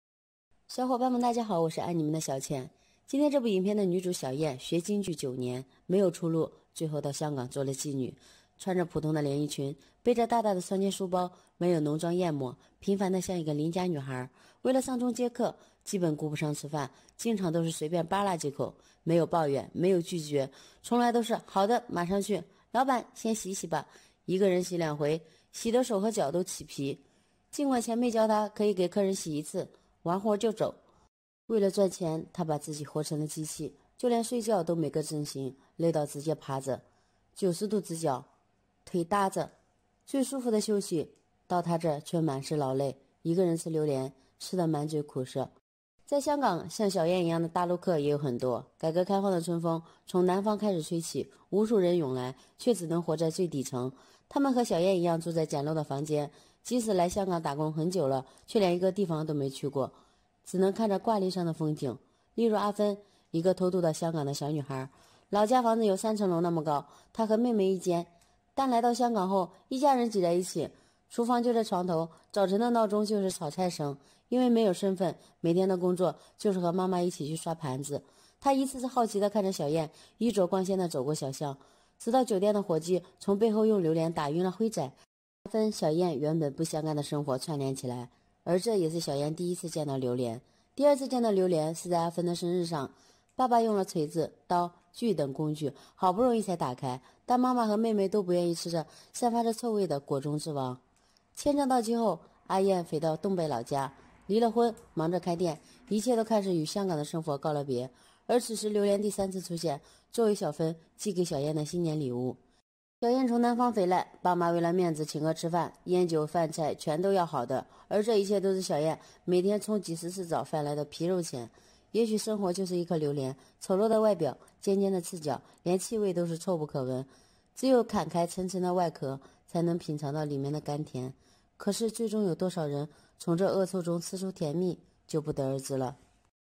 学京剧九年，却没有找到出路，不得已去香港做了妓女，每天给客人洗澡洗到手脚都起皮，最后签证到期回了家，过上了平淡的生活
也许生活就是一颗榴莲，丑陋的外表、尖尖的刺角，连气味都是臭不可闻。
只有砍开层层的外壳，才能品尝到里面的甘甜。